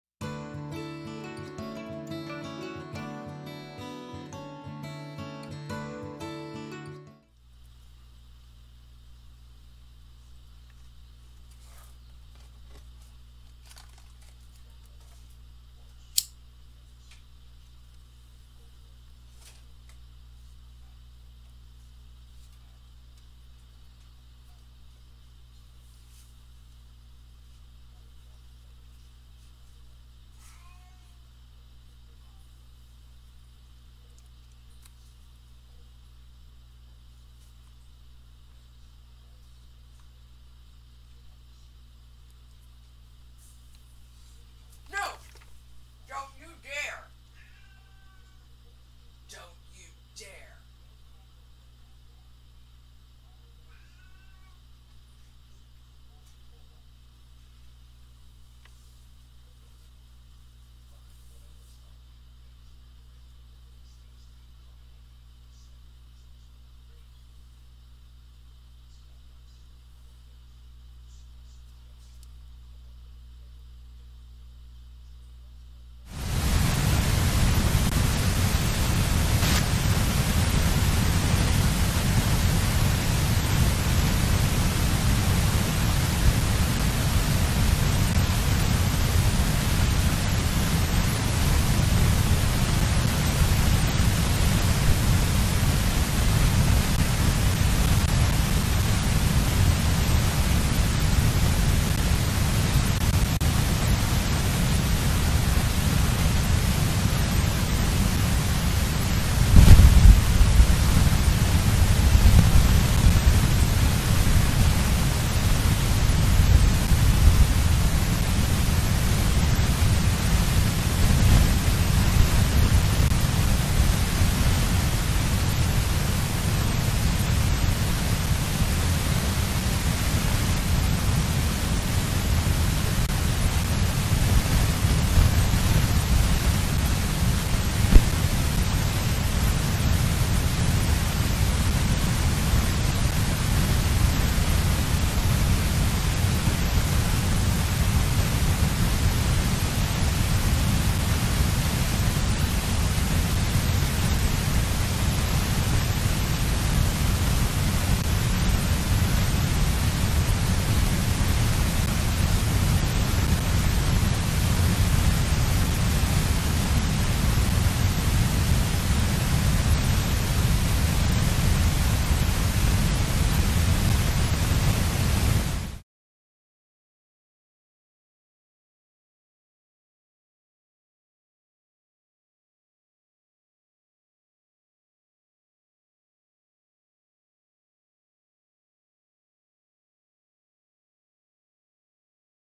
A BIG Fish, Dual Fly-In & Pancake Action - Port Tobacco Bald Eagle Nest of Chandler & Hope (1/29)
The fluffing went well enough, but later in the afternoon Hope decided to get a little pushy. 😂 I guess that's one way to get your nest time!

Screen rec from Port Tobacco Eagle Cam (Jan. 29, 2024)